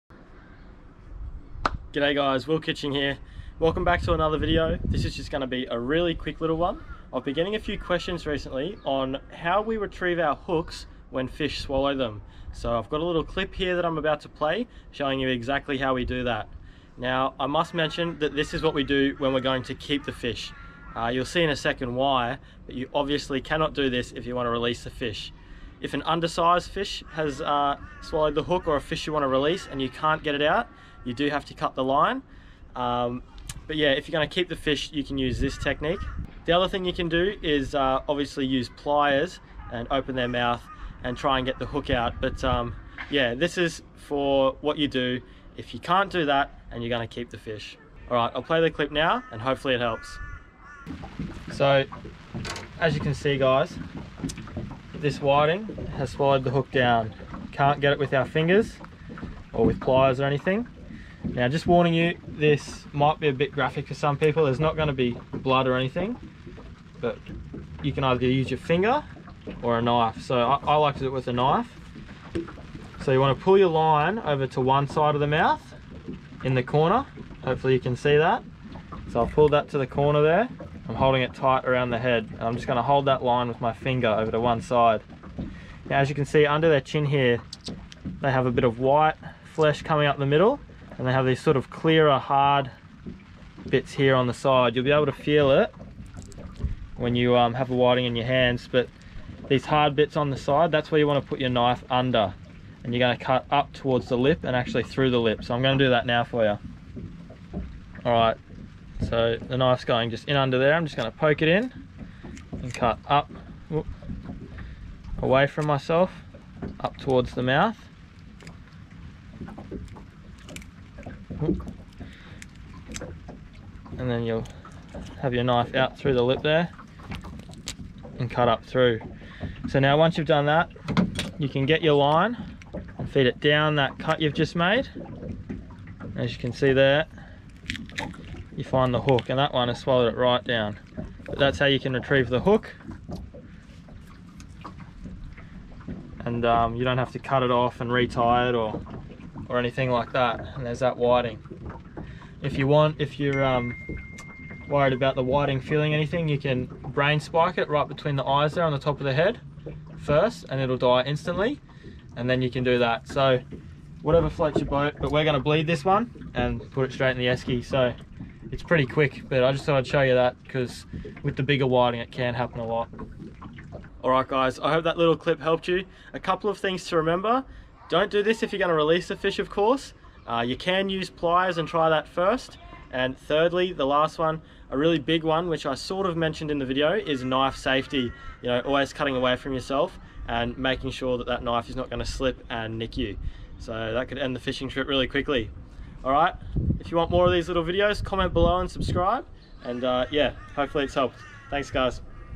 How to remove a swallowed fish hook - LIVE DEMONSTRATION!
This short video shows how to remove fishing hooks when they have been swallowed deep down by the fish. It includes a live demonstration.

Please be careful if you choose to use a knife!

Feel free to check out some of the other videos on my channel with more fishing action!

Writer for- Fish and Boat Magazine